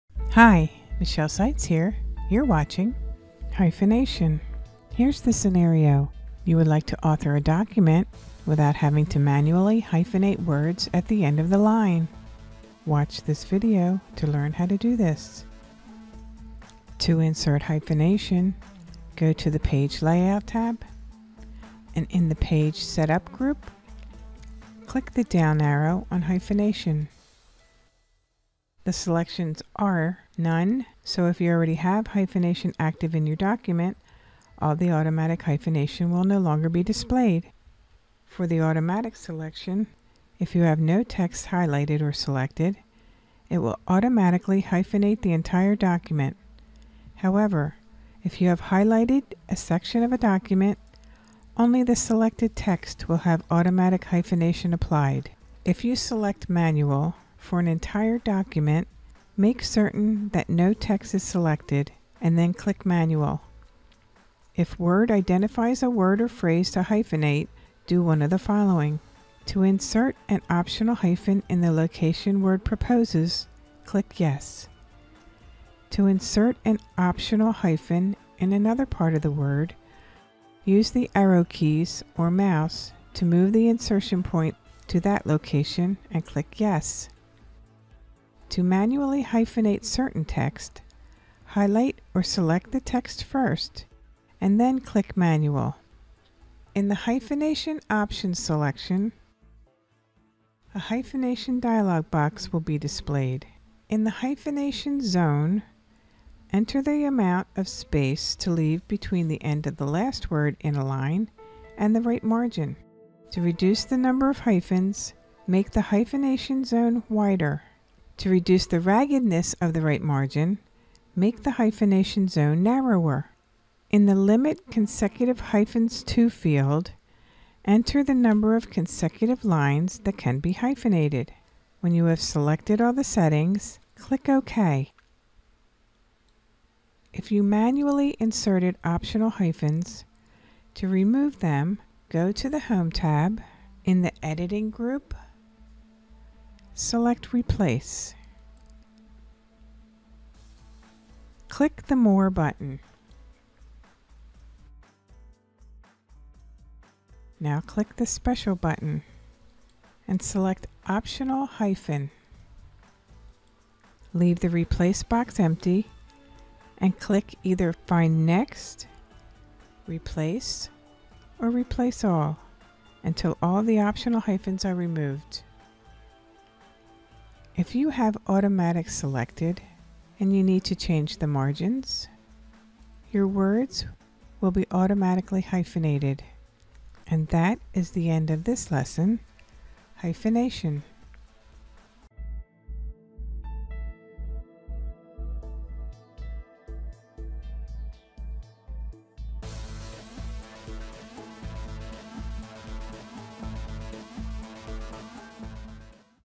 Word 2010 Hyphenation- Add Automatic Hyphenation To Your Documents
Learn how to add and delete hyphenation in your documents. When you change the margins, if you have hyphenation enabled, the words will automatically hyphenate when needed.